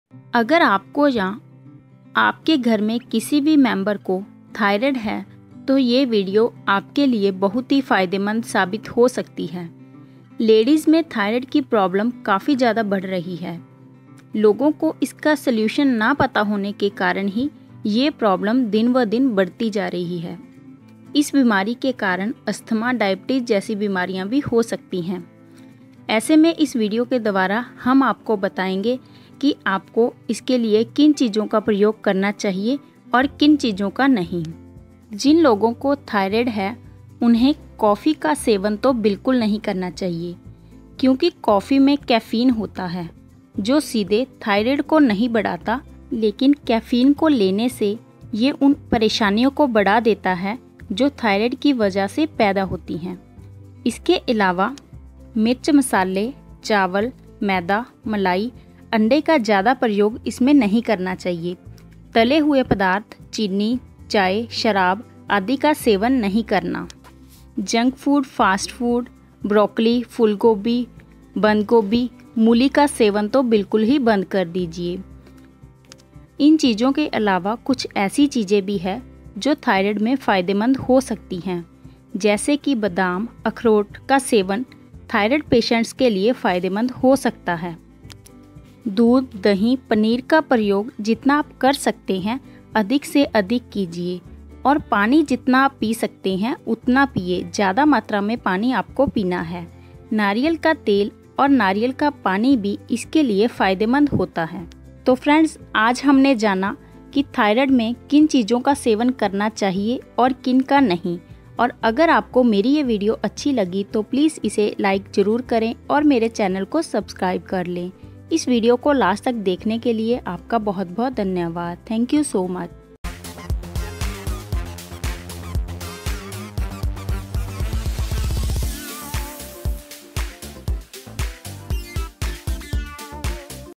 थाइराइड है तो भूल कर भी न चखे ये 4 चीजे वरना पढ़ सकते है लेने के देने - जरूर देखे!
थाइराइड है तो भूल कर भी न चखे ये 4 चीजे वरना पढ़ सकते है लेने के देने - जरूर देखे! (Hindi)
Thanks..